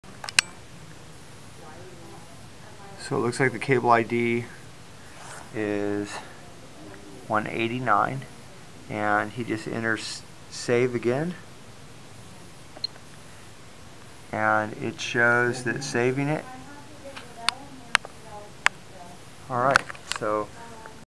Data Cable Analyzer - FLUKE
A quick functional over view of many of the Fluke cable analyzers abilities